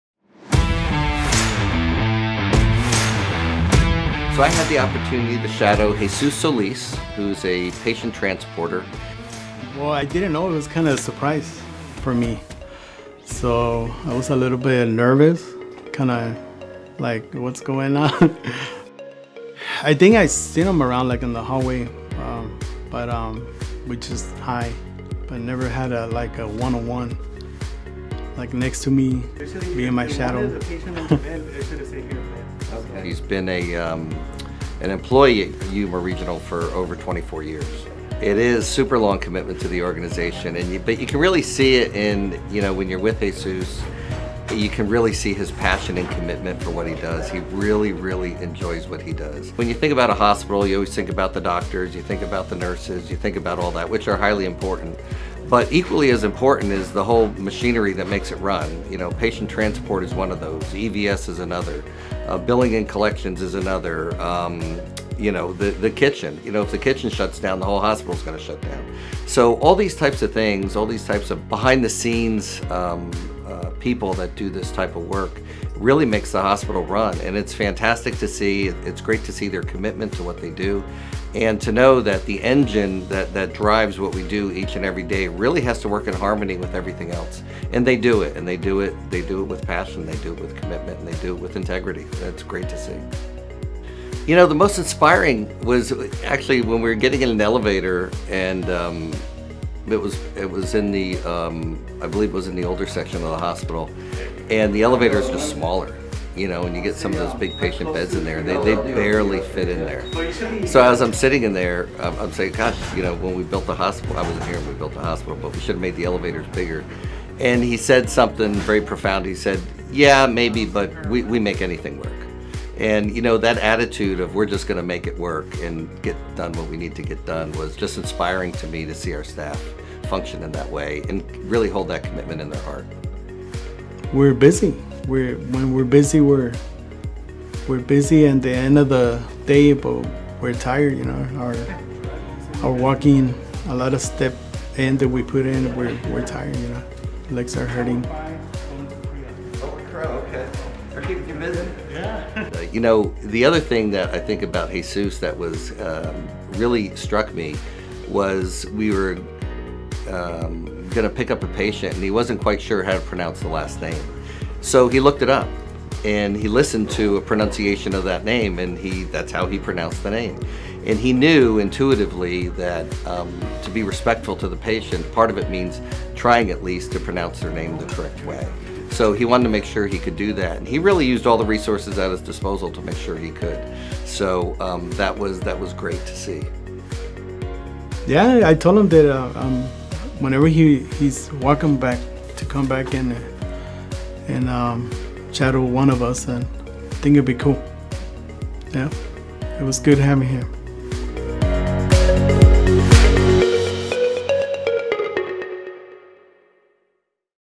Yuma Regional Medical Center Not So Undercover Boss - Episode 3
In this episode of YRMC's Not So Undercover Boss, Jesus Solis from the Patient Transport team shows our President and CEO, Dr. Robert Trenschel, how to get patients to all the places they need to go around the hospital. Together, they hustle to keep up with the brisk YRMC pace. Thank you, Jesus, for the personal touch you provide to the people we serve.